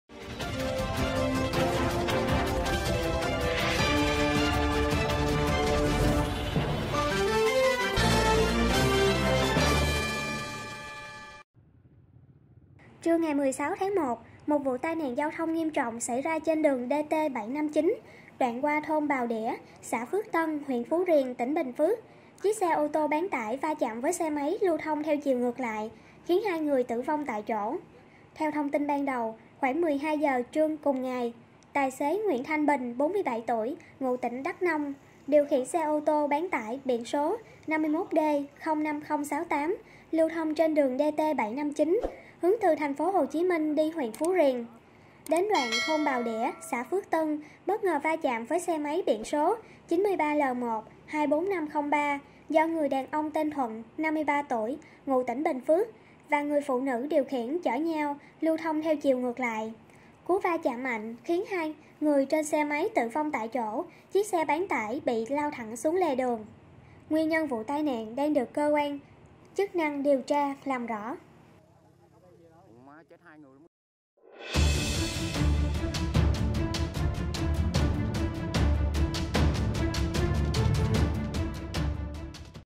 XE BÁN TẢI VA CHẠM XE MÁY, 2 NGƯỜI TỬ VONG TẠI CHỖ
Trưa 16/1, một vụ tai nạn giao thông nghiêm trọng xảy ra trên đường ĐT.759 (đoạn qua thôn Bàu Đỉa, xã Phước Tân, huyện Phú Riềng, tỉnh Bình Phước), chiếc xe ô tô bán tải va chạm với xe máy lưu thông theo chiều ngược lại khiến 2 người tử vong tại chỗ.